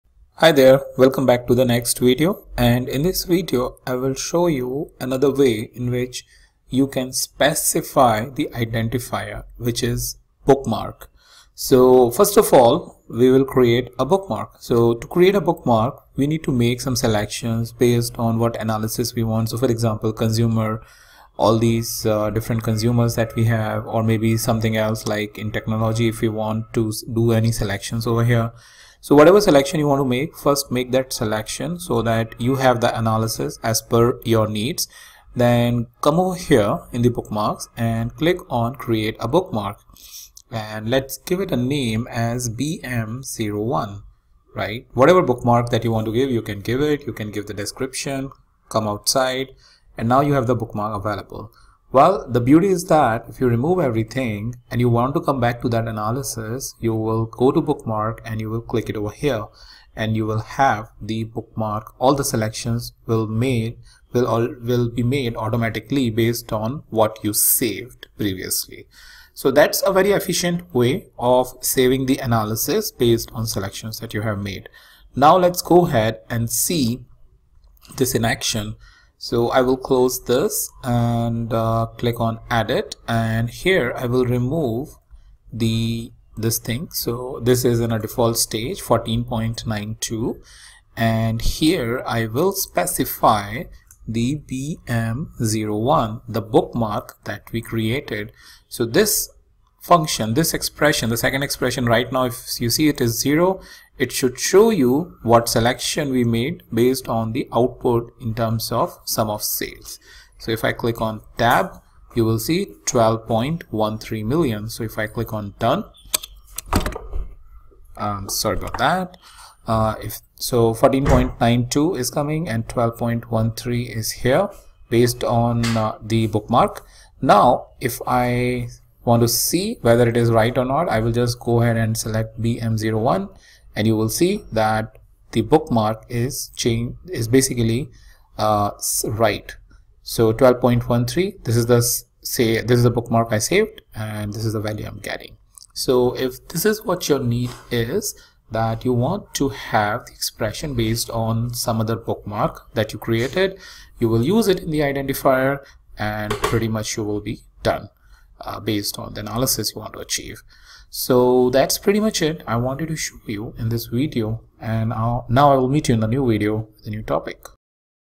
Qlik sense Tutorials - Qlik Sense Set Analysis - - Using Bookmark as Identifier
In this qliksense set analysis tutorial video I have talked about how you can use bookmarks as identifier which is helpful in picking up all the selection values which have been saved in bookmark and then can be used within set analysis.

qlik sense tips and tricks,
qlik sense tricks,